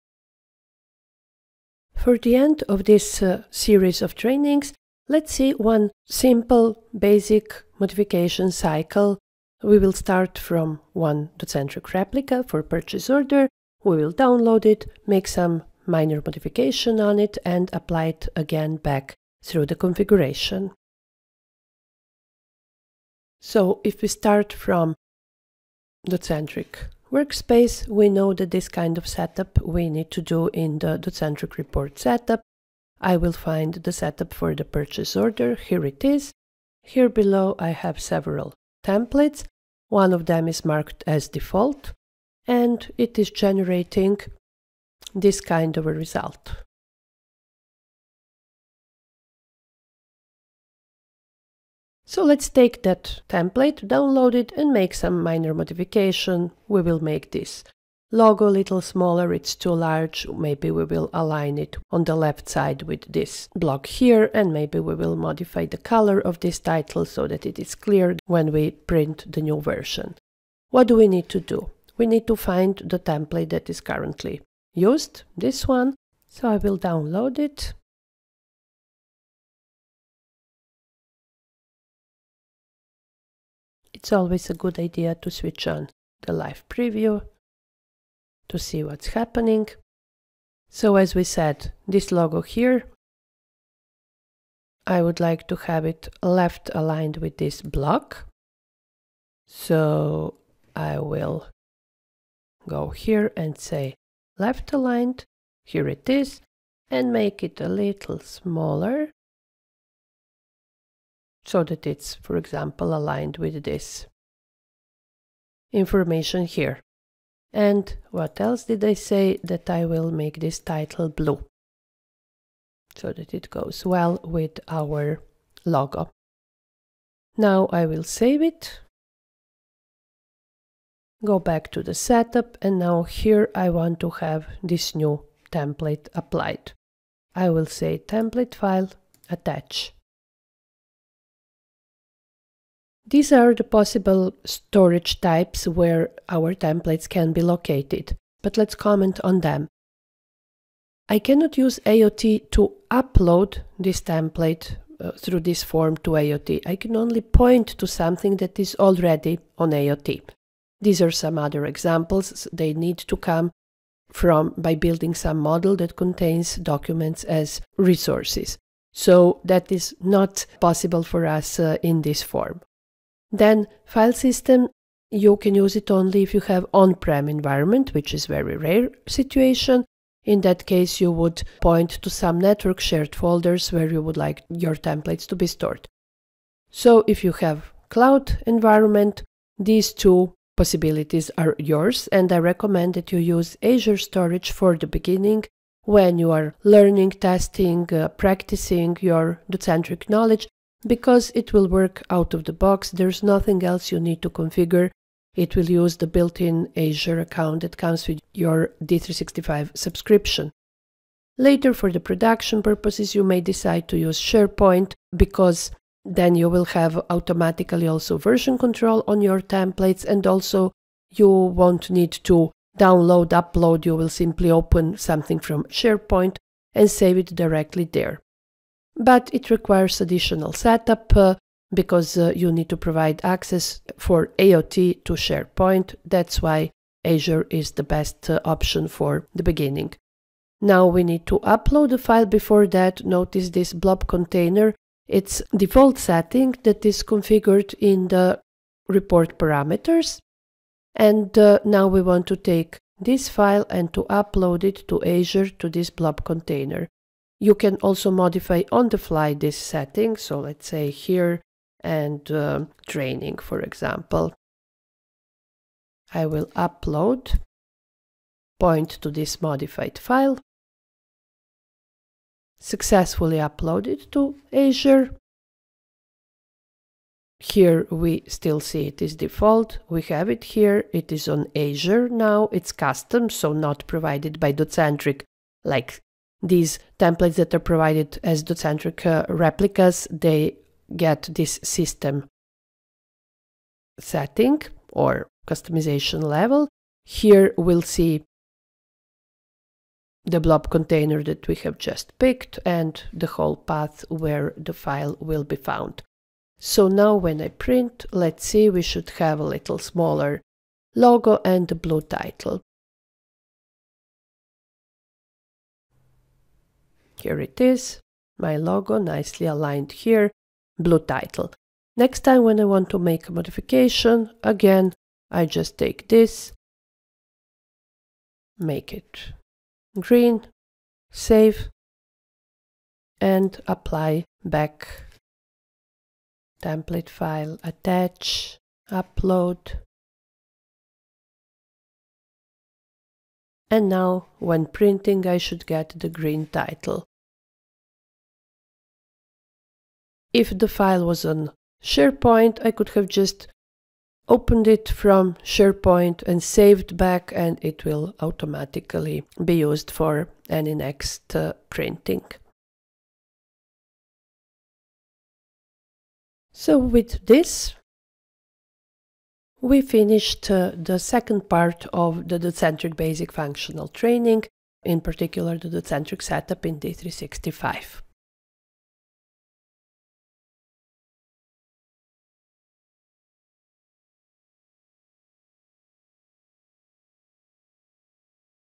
Basic setup: Template modification cycle
Watch this video to learn how to start from the Docentric replica design, download the replica template, modify it, and apply the modified template in a matter of minutes.